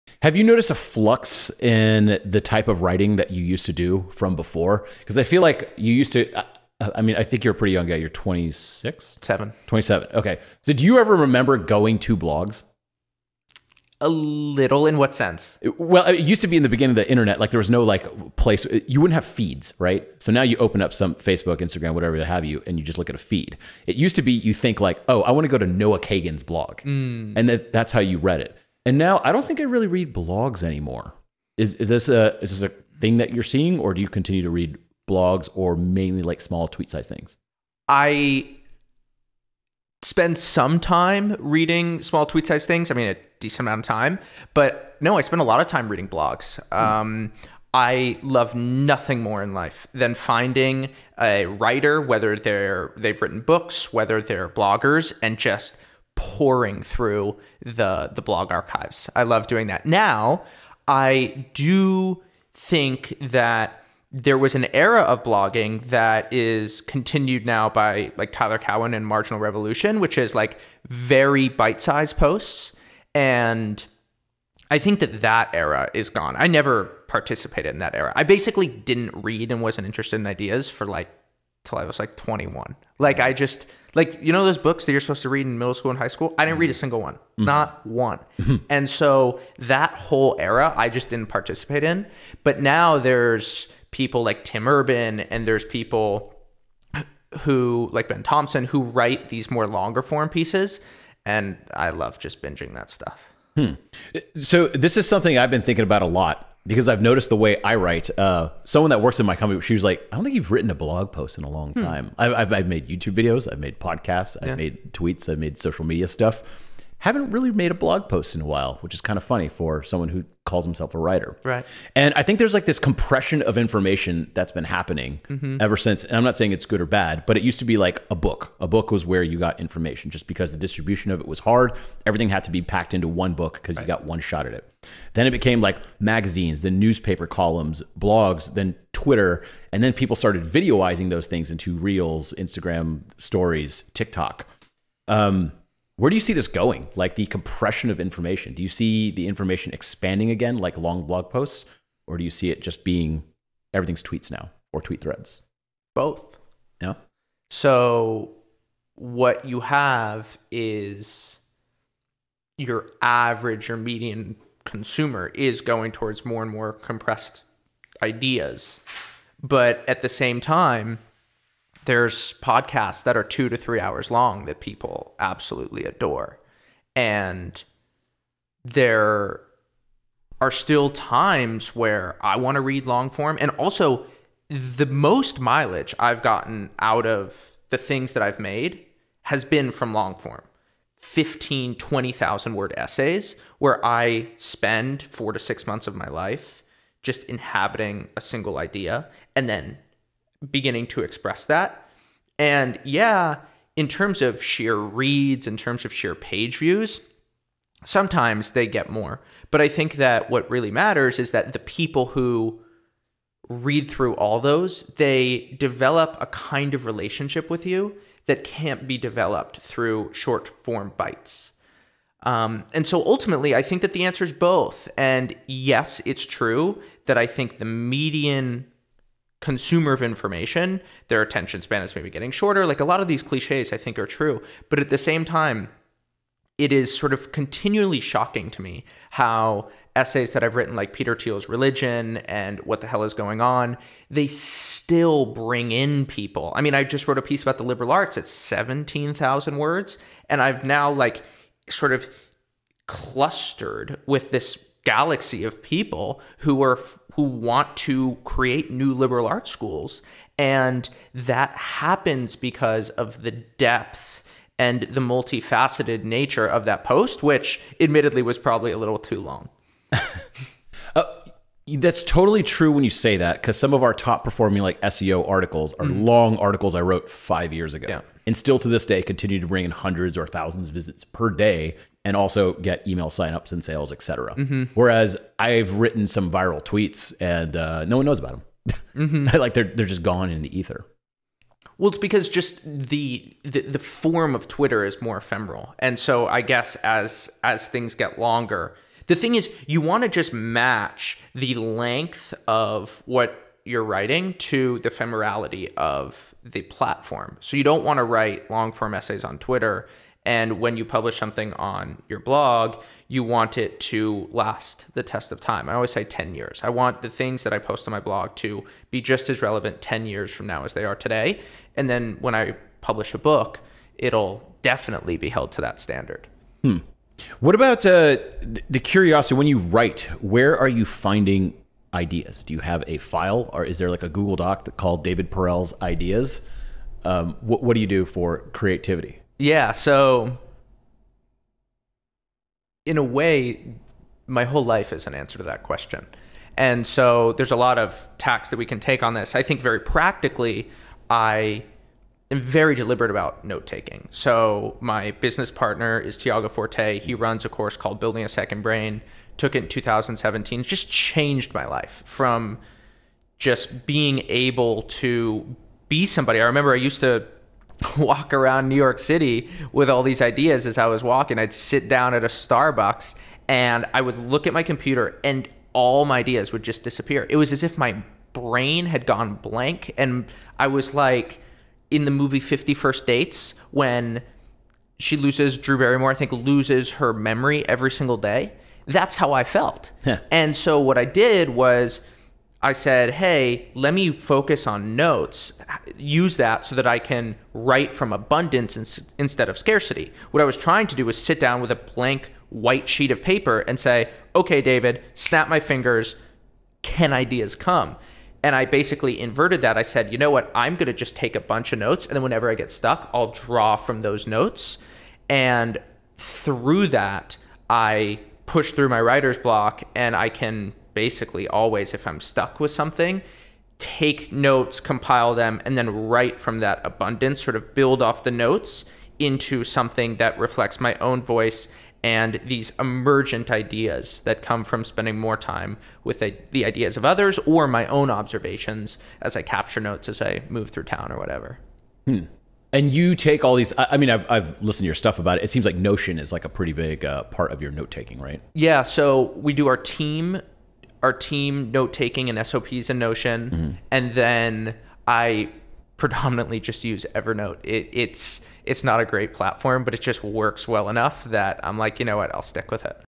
Thoughts On Writing (The "compression" of information + the future of writing)
David Perell and Neville Medhora talk about writing and and how it's changed over the years.

✉️ Signup to our email newsletter:
• 85% of my writing goes out via email!
• Get our full length interviews and clips sent directly to your inbox.